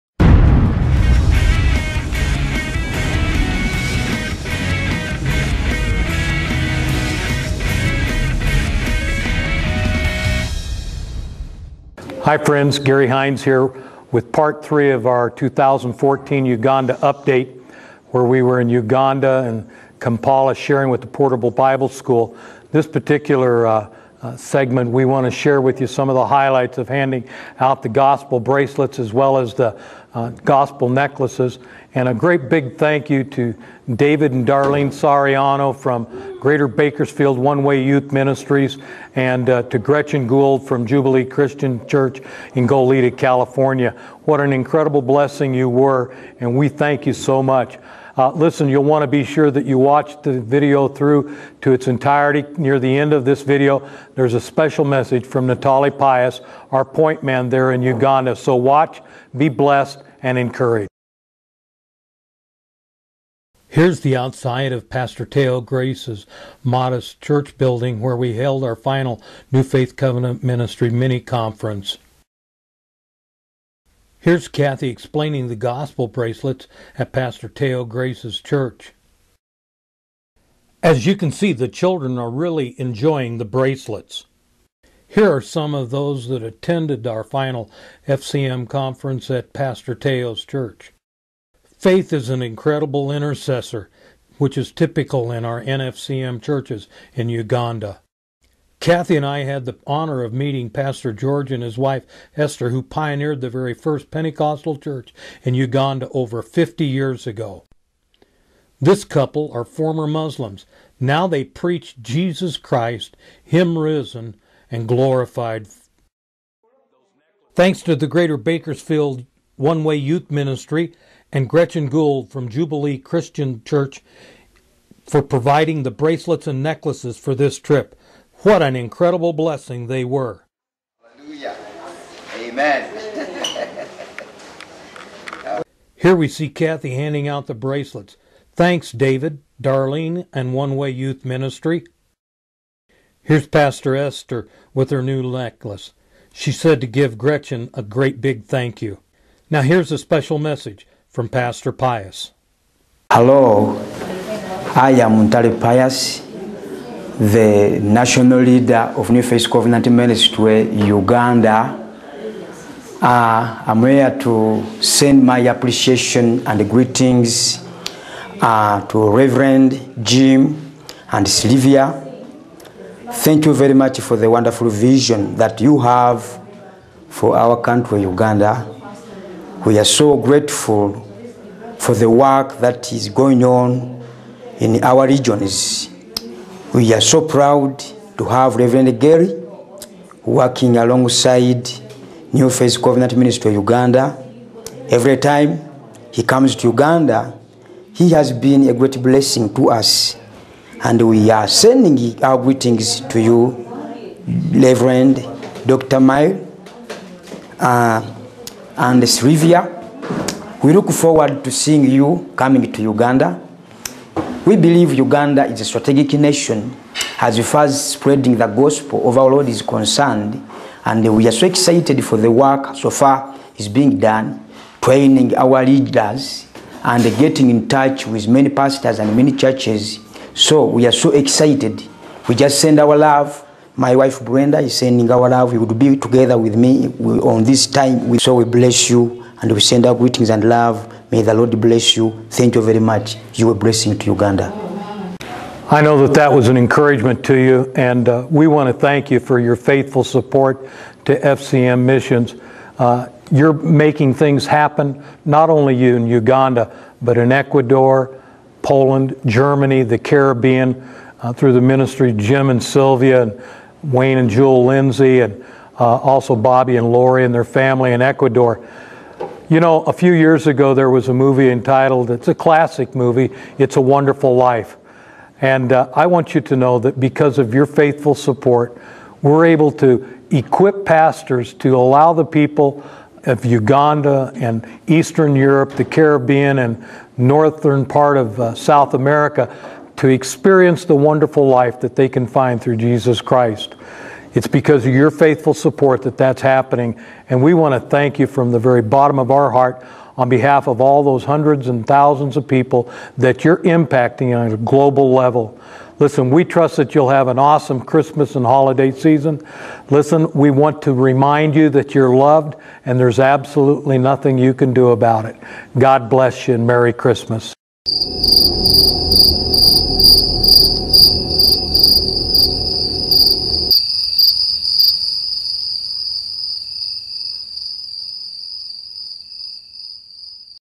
Pt 3 Uganda 2014
Part 3 Uganda 2014 Video.  Highlighting distribution of Gospel bracelets and necklaces, as well as a message from Ntale Pius.